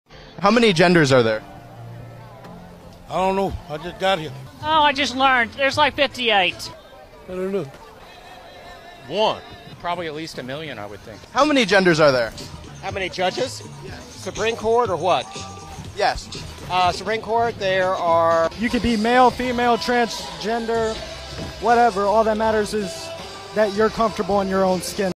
🎲how many genders
Characters in order are Amari (dog), Halrug, Peri, Damir, Tanya, Ebony, Juniper and Magnus from a campaign im in called EDL. Everyone but Amari is owned by my pals.